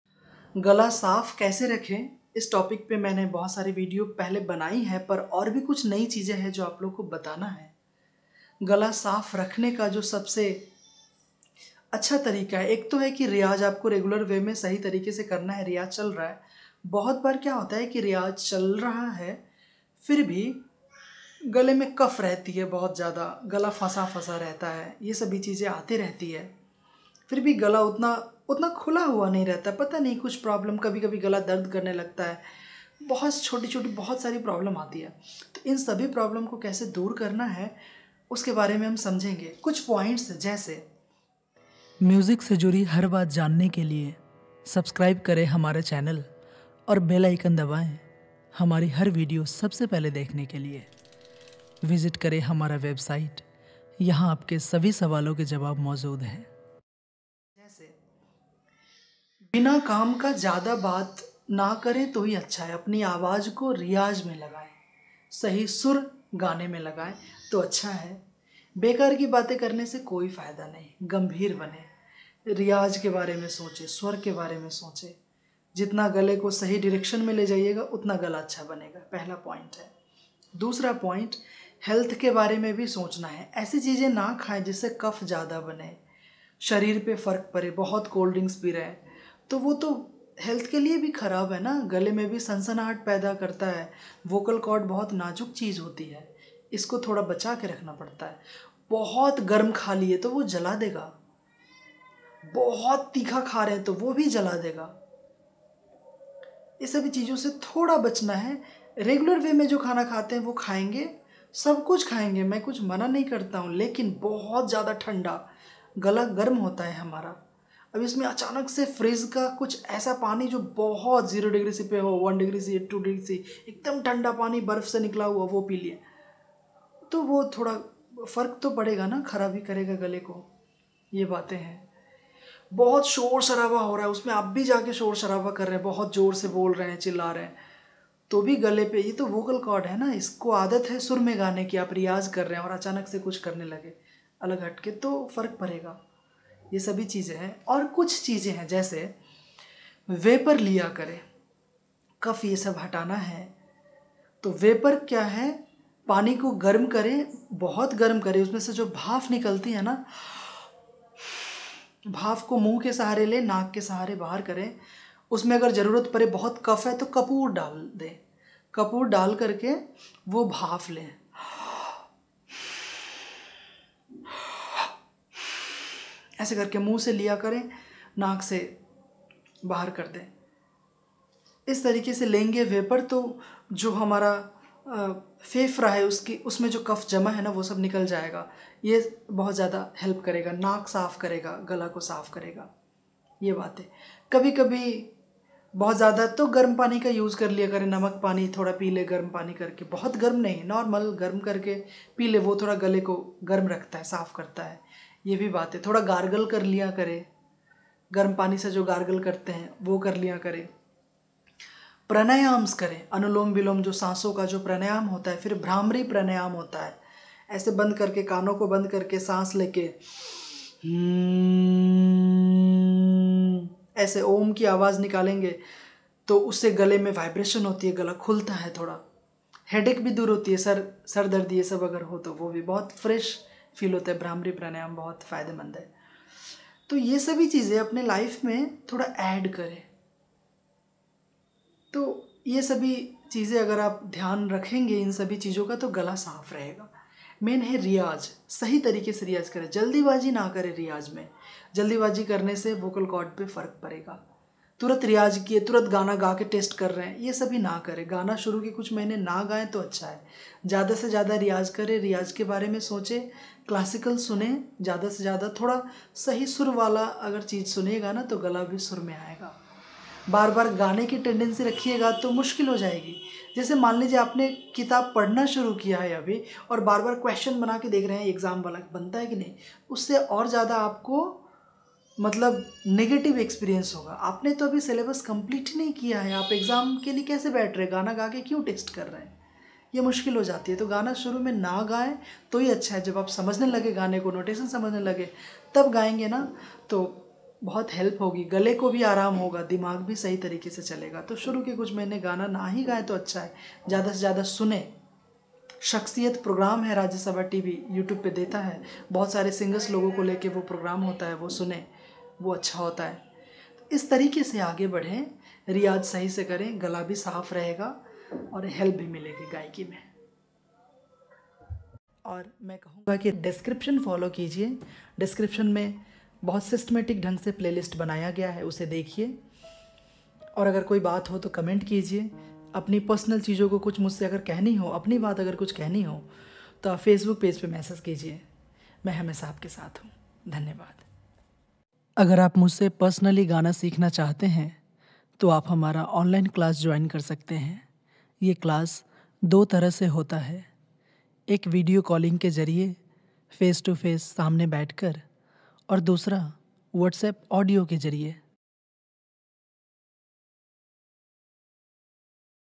गला साफ रखने का अचूक उपाए Gala Saaf Rakhna मेरा अनुभव | Indian Music ART
Gala saaf rakhne ka achuk upaye"
Aksar log roj riyaz karte hain fir bhi gala puri tarah saaf nahi reh pata hai, isliye maine apna anubhav aaplogo ke sath share kiya hai aur upaye bhibataya hai ki kaise gala saaf rakha jaye...

by Rohit kumar Singh